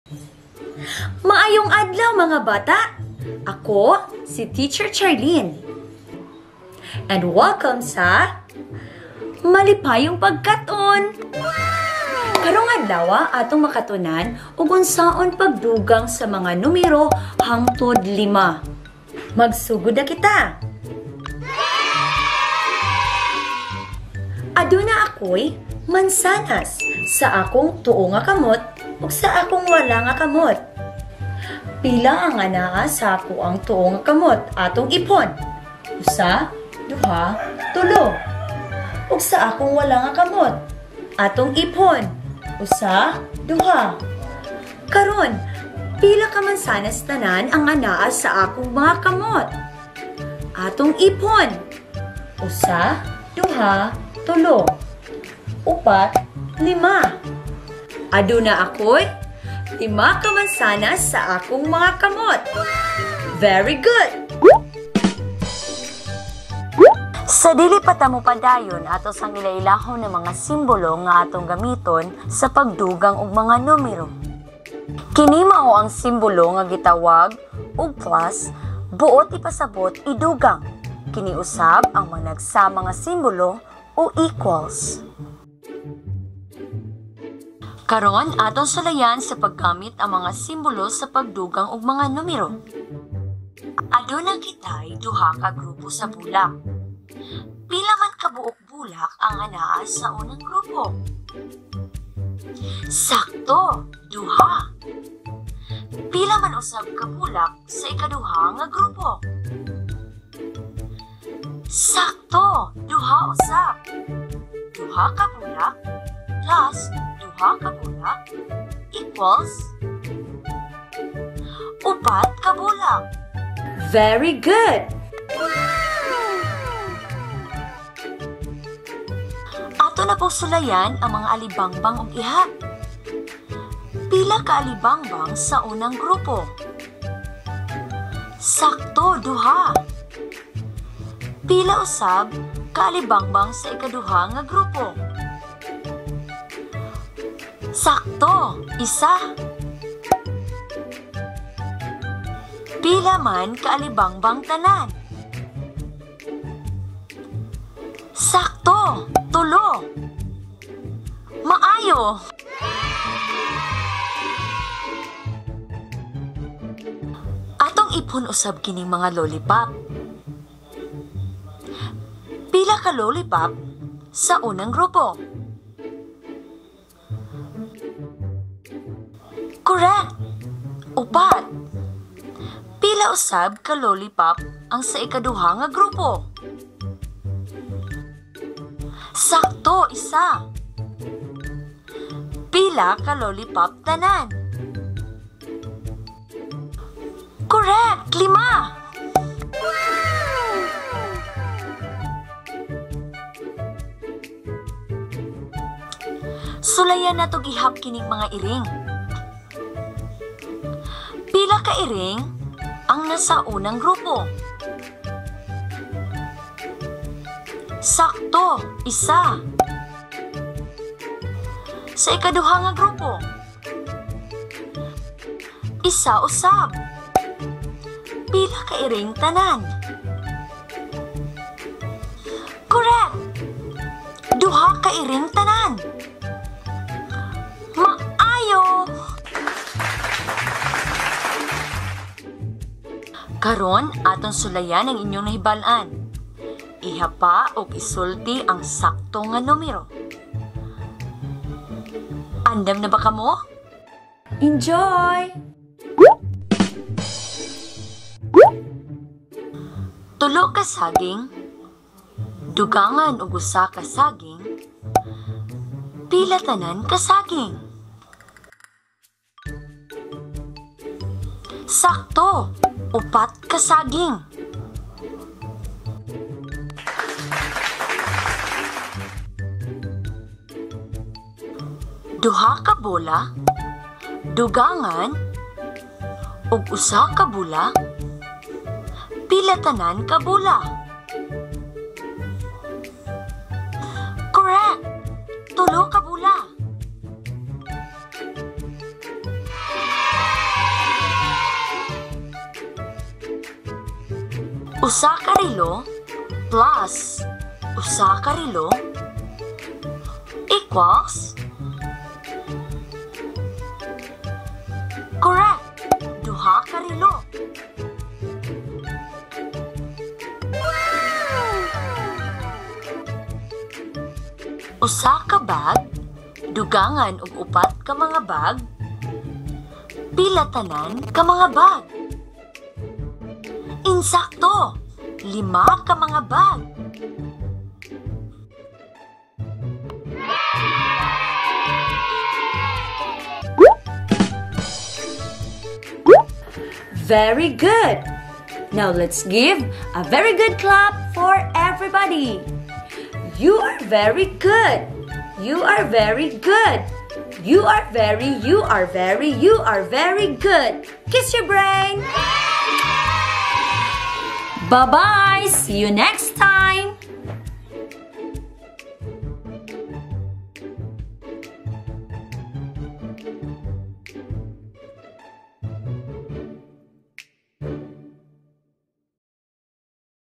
ADDING NUMBERS UP TO FIVE | BISAYA VERSION | CHARLENE ALDUHESA PENOLIAD |VIRTUAL LEARNING
Topic: Adding Numbers Up To Five / Pagdugang ug Numero Hangtud Lima
Teacher: Charlene Alduhesa Penoliad
School: Sumirap Central School
Division: Tangub City